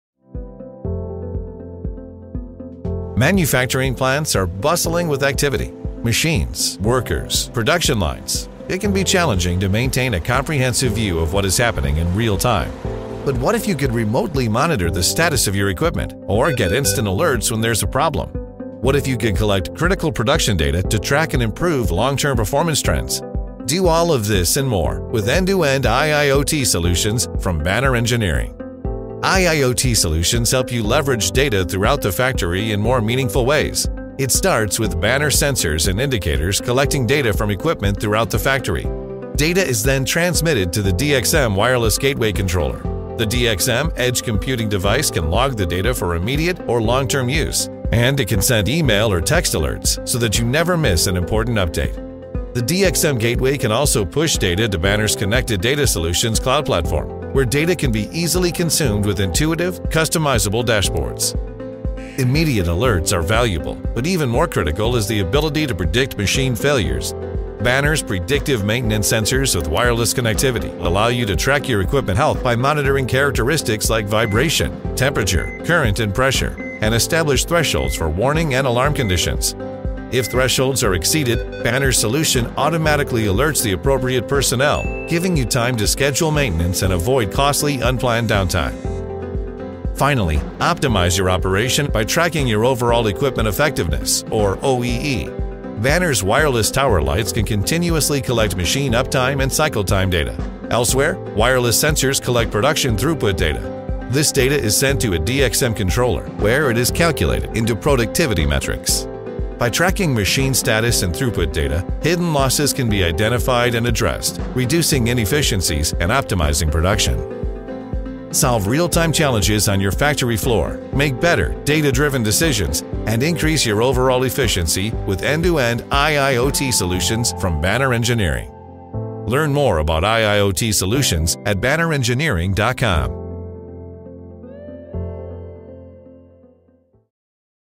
Endüstri 4.0 Çözümleri
Banner’ın sunduğu Endüstri 4.0 çözümleriyle veriye dayalı kararlar alarak üretim verimliliğinizi en üst seviyeye taşıyabilirsiniz.